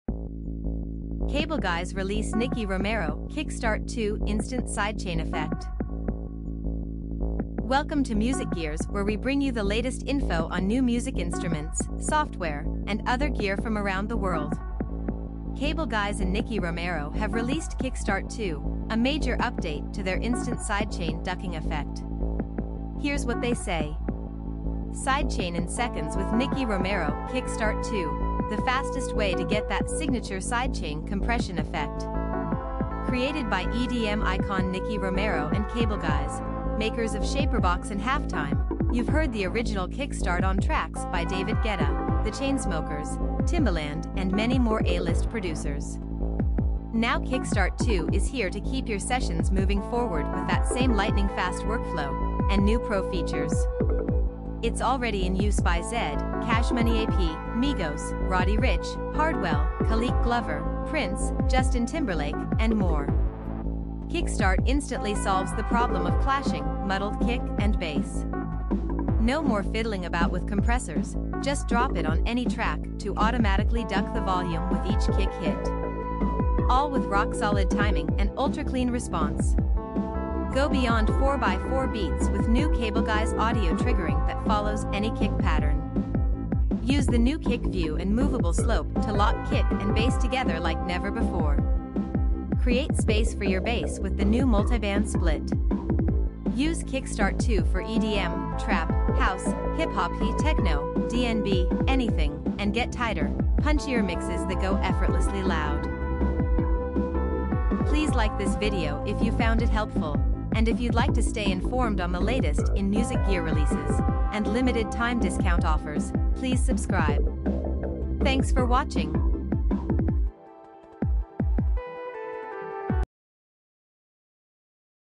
Cableguys release Nicky Romero Kickstart 2 - Instant Sidechain Effect
Cableguys and Nicky Romero have released Kickstart 2, a major update to their instant sidechain ducking effect.

Here's what they say:

Sidechain in seconds with Nicky Romero Kickstart 2 – the fastest way to get that signature sidechain compression effect.

Created by EDM icon Nicky Romero and Cableguys (makers of ShaperBox and HalfTime), you've heard the original Kickstart on tracks by David Guetta, The Chainsmokers, Timbaland and many more A-list producers.

Now Kickstart 2 is here to keep your sessions moving forward with that same lightning-fast workflow, and new pro features. It's already in use by Zedd, CashMoneyAp (Migos, Roddy Ricch), Hardwell, Khaliq Glover (Prince, Justin Timberlake), and more.

Kickstart instantly solves the problem of clashing, muddled kick and bass. No more fiddling about with compressors – just drop it on any track to automatically duck the volume with each kick hit. All with rock-solid timing and ultra-clean response.

Go beyond 4x4 beats with new Cableguys audio triggering that follows any kick pattern. Use the new kick view and movable slope to lock kick and bass together like never before. Create space for your bass with the new multiband split.

Use Kickstart 2 for EDM, trap, house, hip-hop, techno, DnB – anything – and get tighter, punchier mixes that go effortlessly loud.